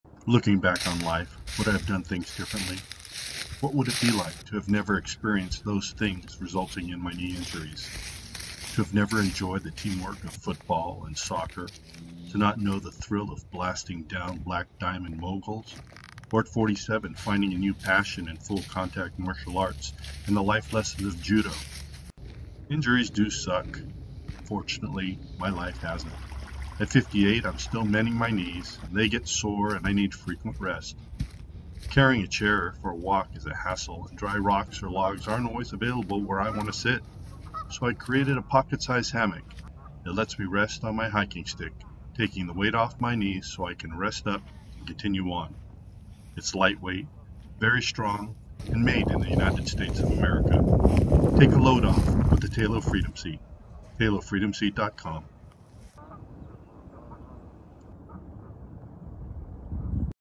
Resting Aid
Ultra-Light Seat for Walking Sticks and Hiking/Trekking Poles

Rest and Relief when and where you want with the Ultra-Light, Ultra-Strong, Ultra-Easy-to-Use seat accessory for walking sticks and hiking/trekking poles.

Take a load off with the TALO Freedom Seat by Maximum Win LLC, Innovations for the Outdoor Enthusiast.